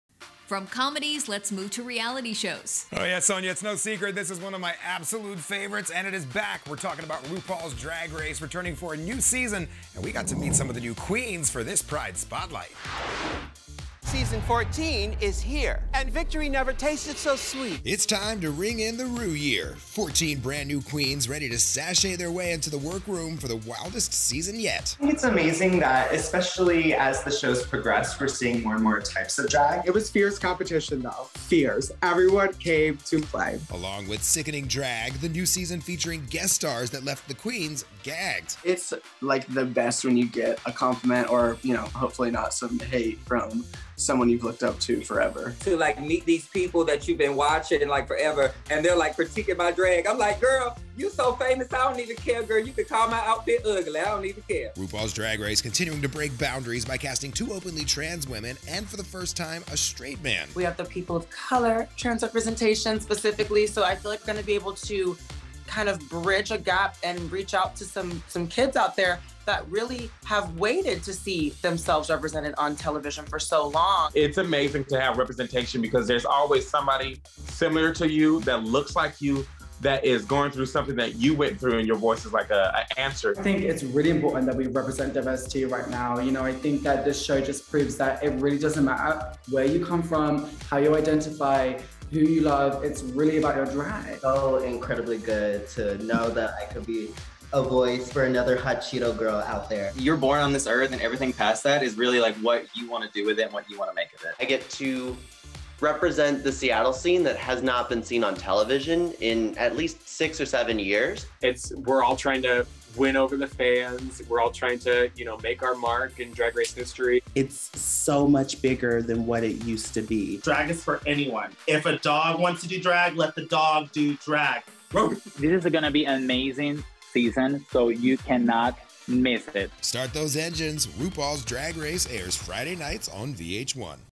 The Queens Of 'RuPaul's Drag Race' Spill On The Most Inclusive Season Yet | Celebrity Page
Let the race begin! DragRace is back and the queens of season 14 are sounding off on the new season.